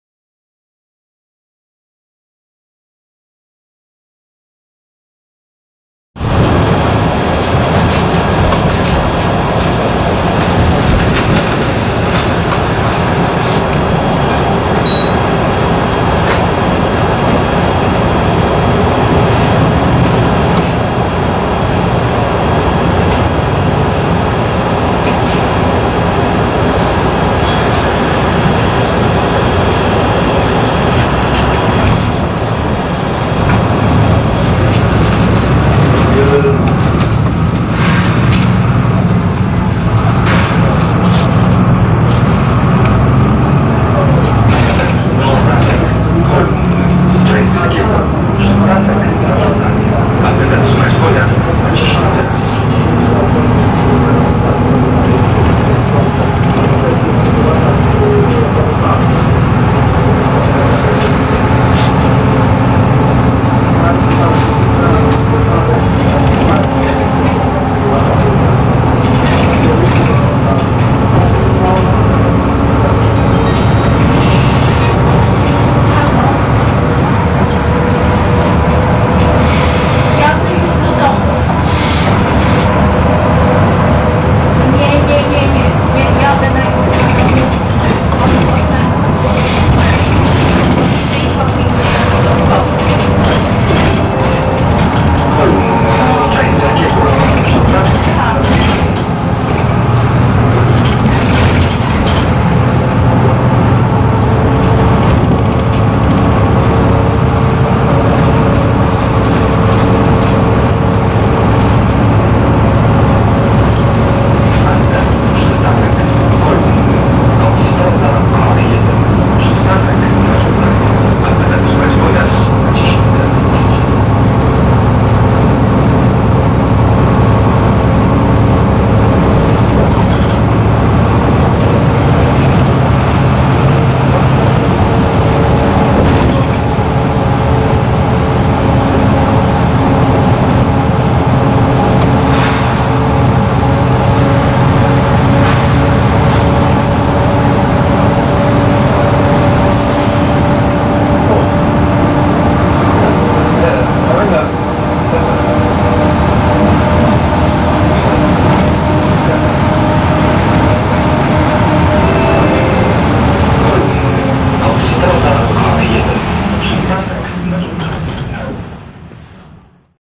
[*] Mercedes Benz Conecto LF #2053 (Linia 92A 🚍) MPK Łódź 🎵ZF🎵 [*]
Linia 92A [Stróża Kolumny - PL.Niepodległości]

Dane Techniczne:
Model Autobusu: Mercedes Benz Conecto LF #2053 [*]
Rok Produkcji: 2010
Rok Wycofania: 2024
Silnik:
Mercedes Benz OM926LA (286 KM)
Skrzynia Biegów:
ZF 6HP502 (6 Biegów)
Zajezdnia: Nowe Sady [EA-2]

Przystanki:
Stróża Kolumny
Kolumny-Czajewskiego NŻ
Kolumny-Autostrada A1 NŻ